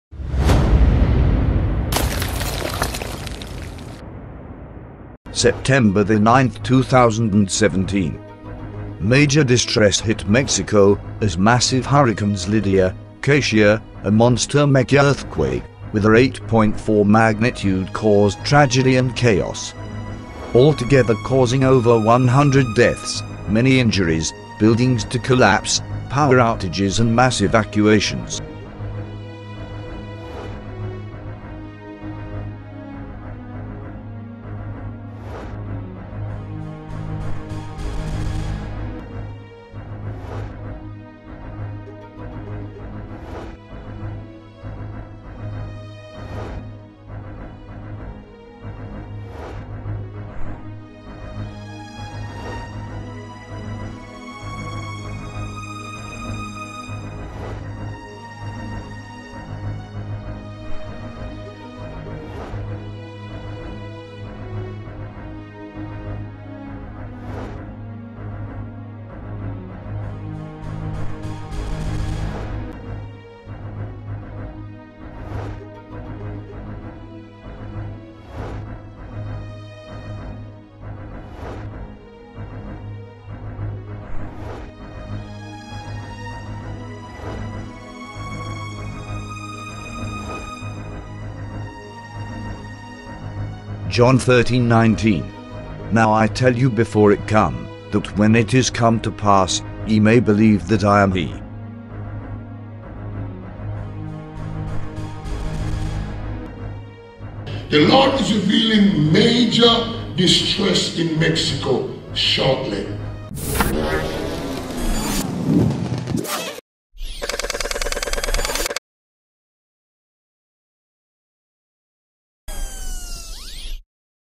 Major DISTRESS ravage MEXICO: MEGA-QUAKE(95) Dead, HURRICANES Katia(2) Lidia(7) Destruction 9.11.17
News Reports
Mexico Earthquake

2:15.13 METEOR EXPLOSION RUSSIA